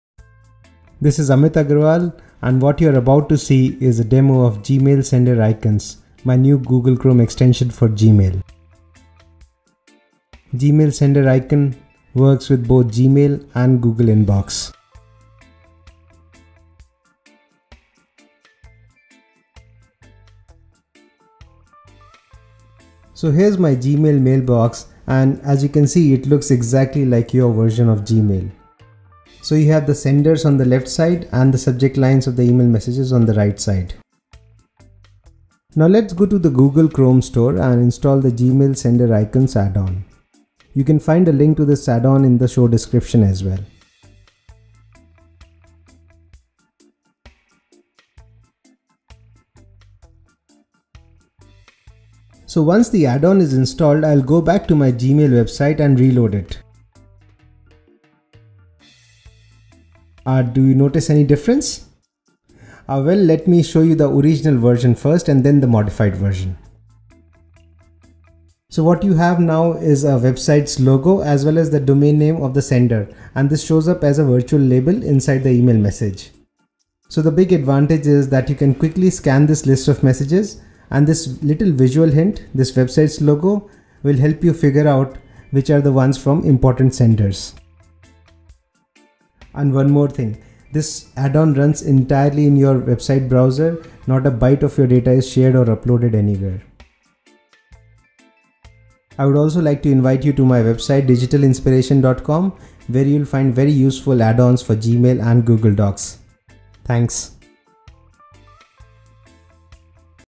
Gmail Sender Icons for Google Chrome
Quickly see the details of the email sender including their website's logo and the domain name within the message list of Gmail.

Useful Google Add-ons for Gmail, Google Sheets and Google Forms